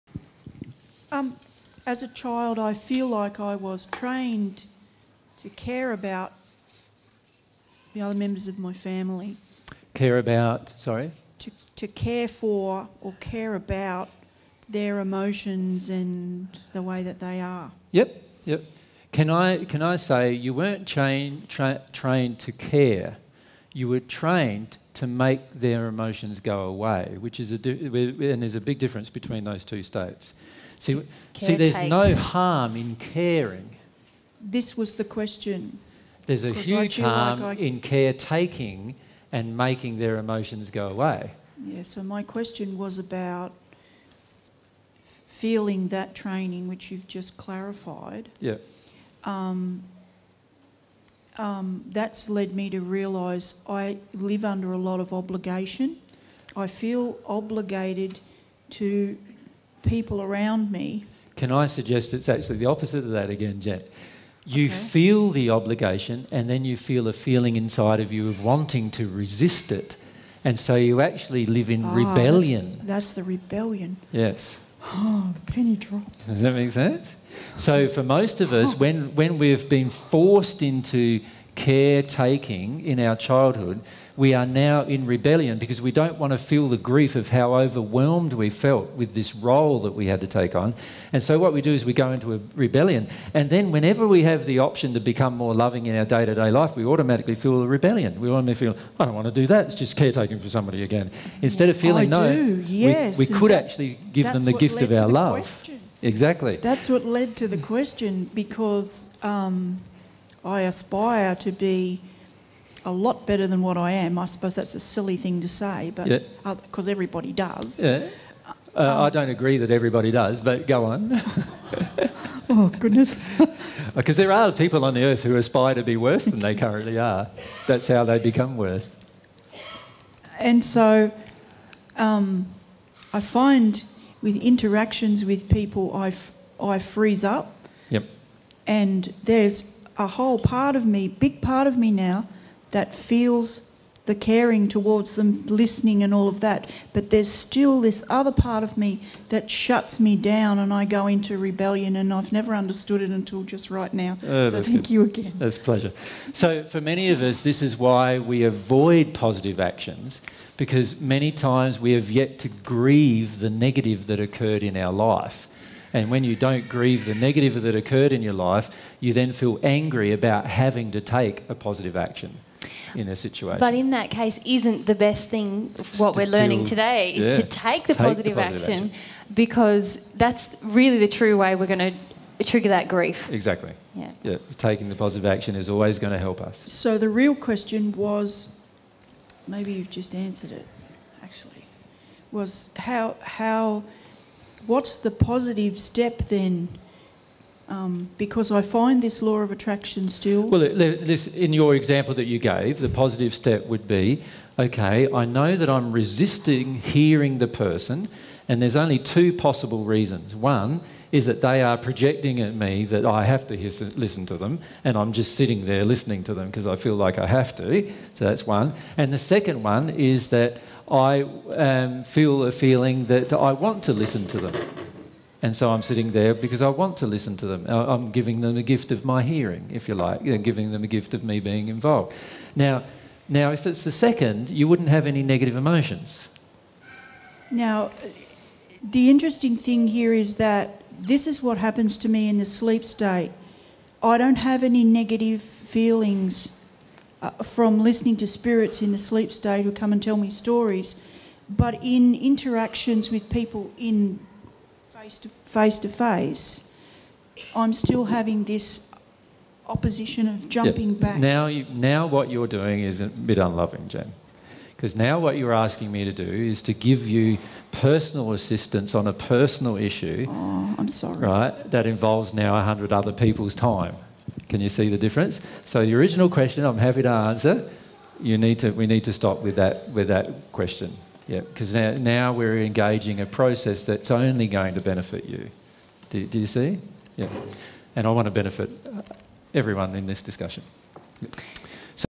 Jesus on Rebellion
Jesus helps a participant explain her resistance to loving others and how to change.

This is a section of the presentation by Jesus and Mary in interview format entitled "20120422 The Human Soul - The Gift Of Free Will S2".